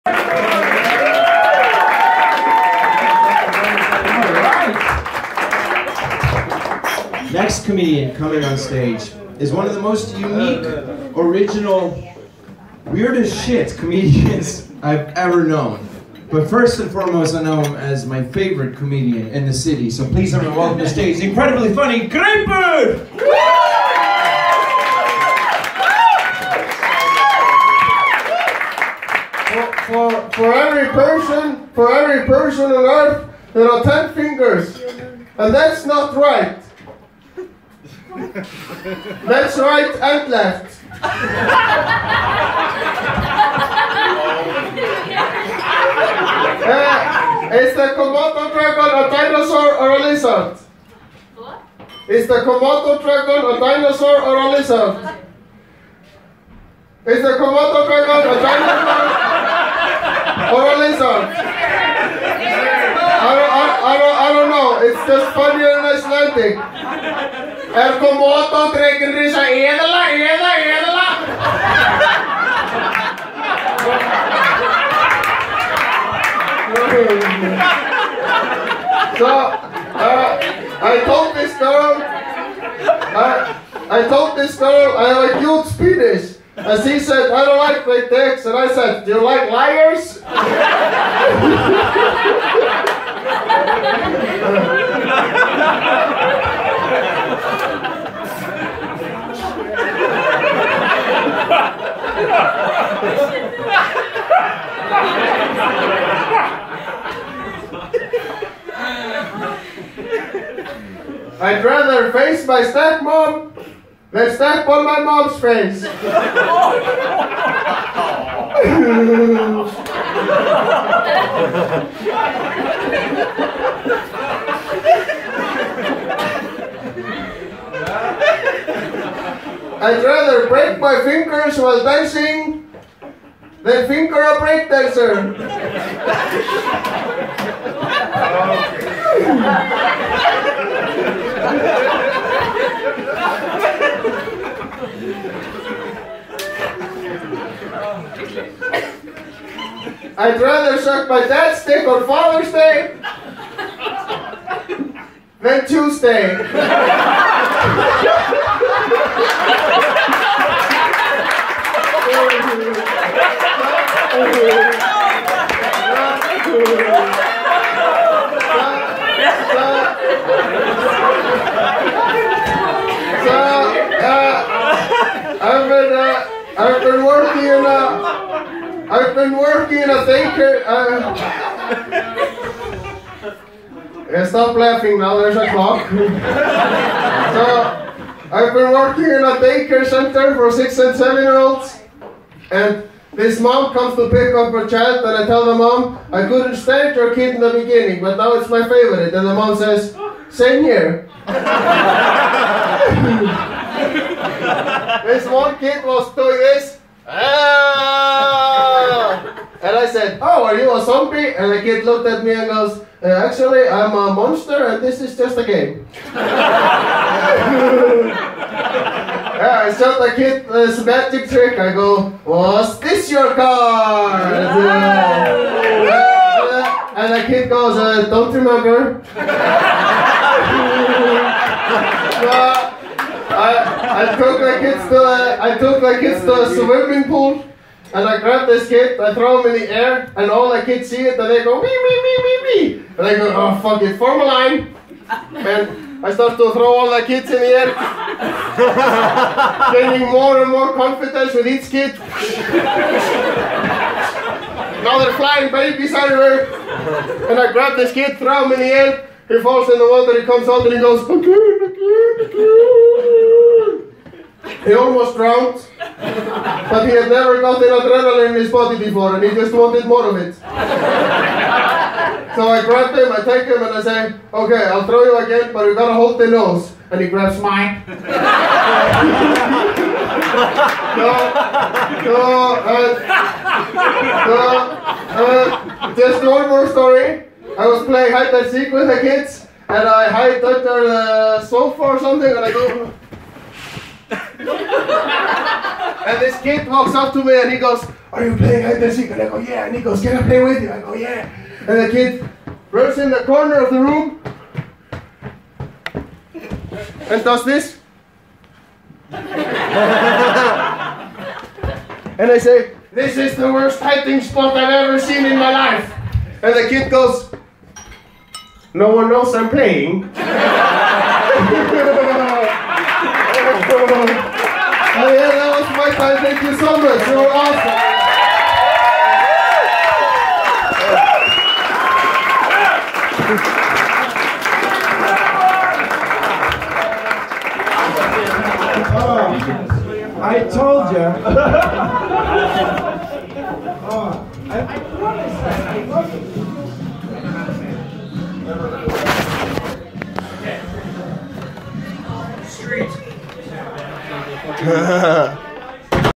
Tight five - Stand up - Greipur
One of my best tight five at the Secret Cellar in Reykjavík.